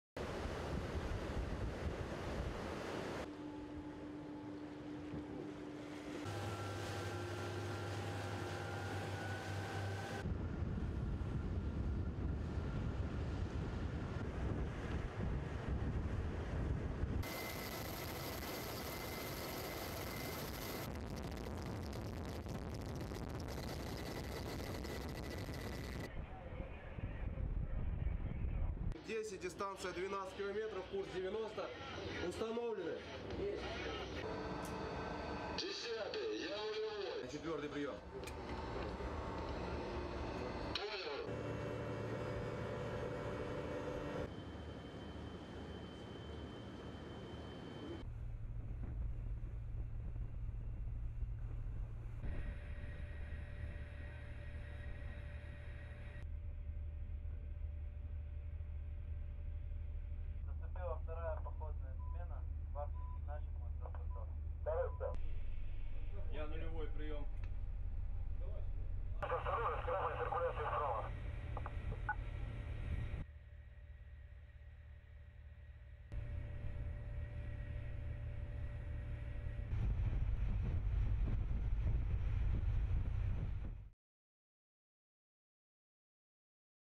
Russia holds military exercises in the Baltic Sea ukraine war
ukraine war

Russia holds military exercises as planned in the Baltic Sea.

The naval tactical groups of Russian Baltic Fleet are now grouped in deaignated areas.

Fourteen NATO countries  are also conducting the BALTOPS 22 naval drills in the Baltic Sea over the next week.

Sweden and Finland are also involed in the naval drills together with the fourteen NATO countries according to a NATO statement.

Will this be the start of WW3? Russia and NATO in the Baltic Sea? Well we will see what happens.

stop russia
slava ukraina
baltops 22
baltic sea